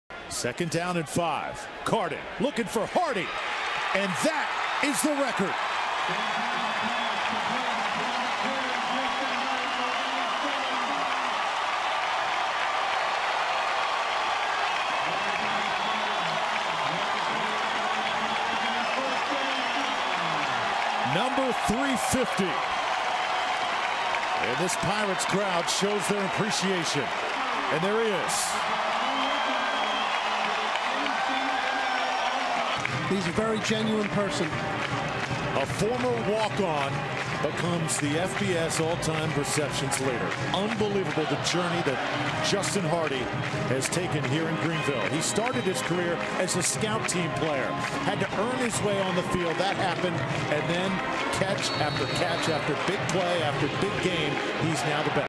Justin Hardy Sets NCAA FBS All-Time Receptions Record
No one has ever caught more passes in FBS history than East Carolina's Justin Hardy. The Senior set the NCAA record for receptions in the Pirates' 34-6 win over Tulane.

He finished with 9 catches in the game, which gives him 355 in his illustrious career. Congratulations Justin!